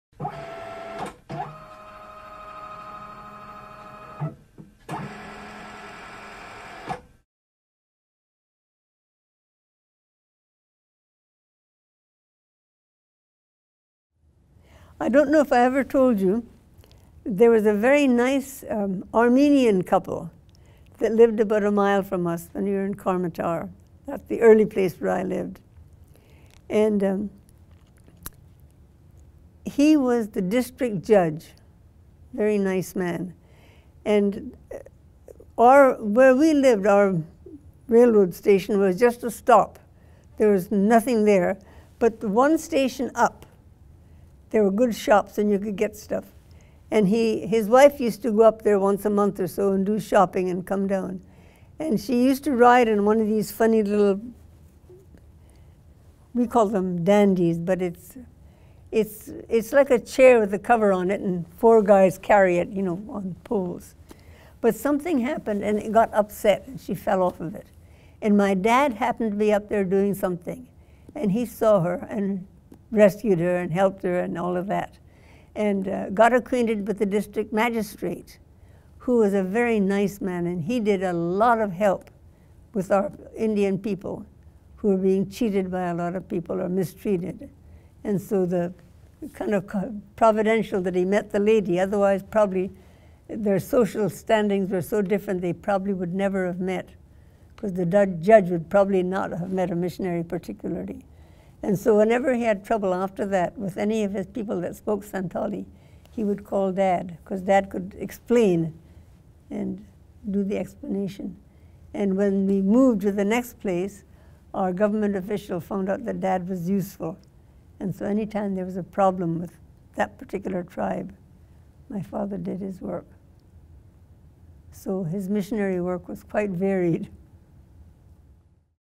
The Upset Litter
When the litter carrying the District Judge’s wife was upset, Robert Borrowdale came to her rescue. Esther Hare shares how this experience was providential for the mission in India.